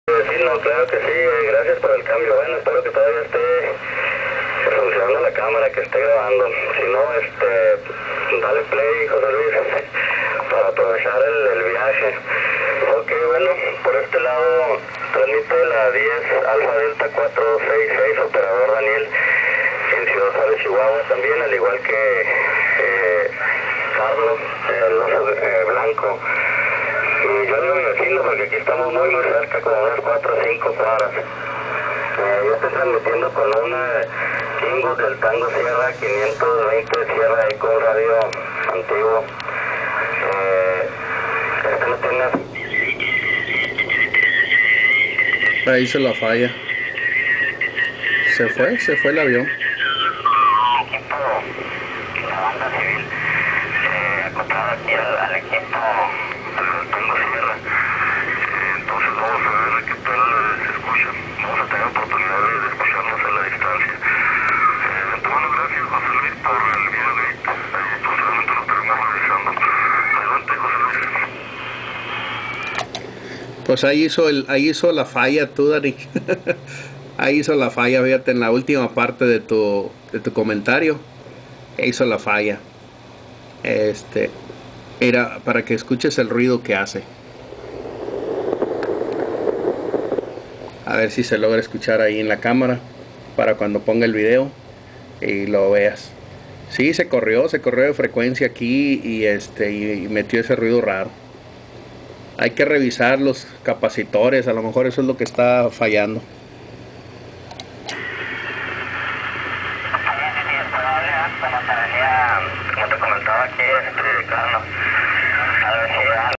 10AD466 Dany. y 10AD113 Jose Luis
Probando el TS820 con una falla rara se movio de frecuencia y comenzo a ocilar raro espero encontrar esa falla que de vez en cuando la hace